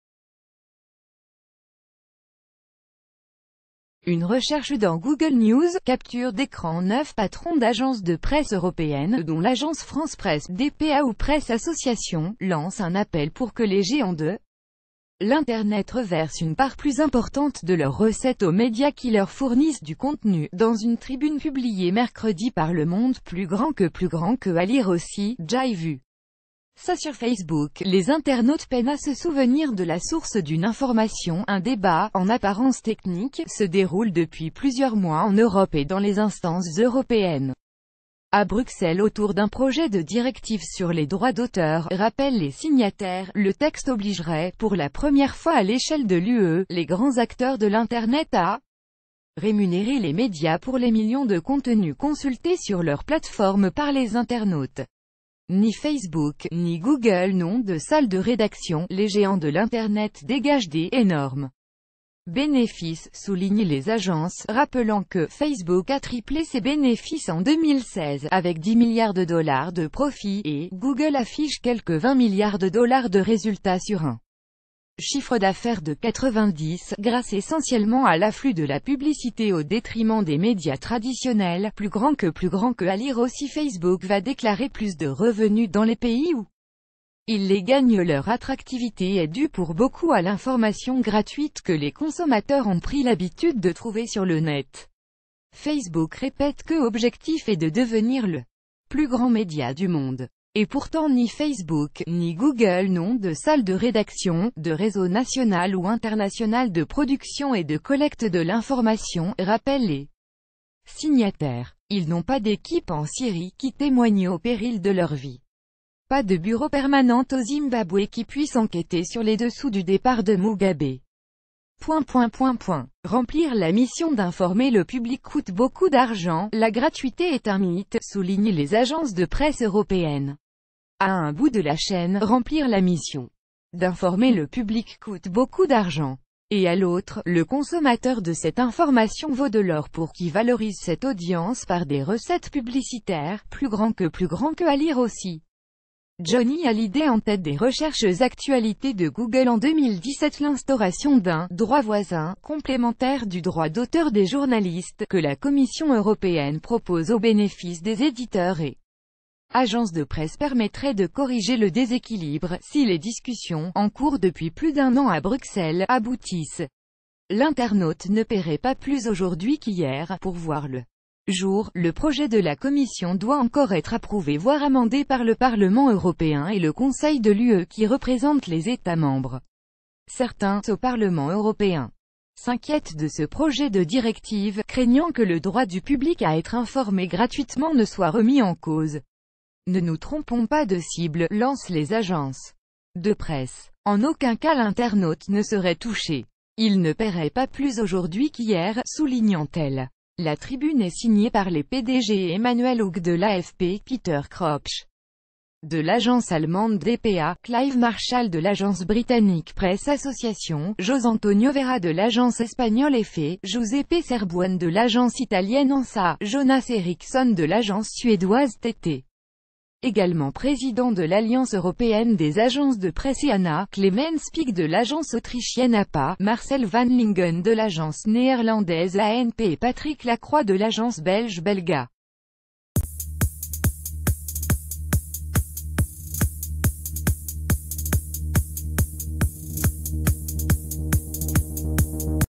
«la gratuité est un mythe»: des agences de presse demandent aux géants de l'internet de passer à la
«la gratuité est un mythe»: des agences de presse demandent aux géants de l'internet de passer à la caisse
Une recherche dans Google News. — Capture décran Neuf patrons d’agences de presse européennes, dont l’Agence France Presse, DPA ou Press Association, lancent un appel pour que les géants de l’Internet...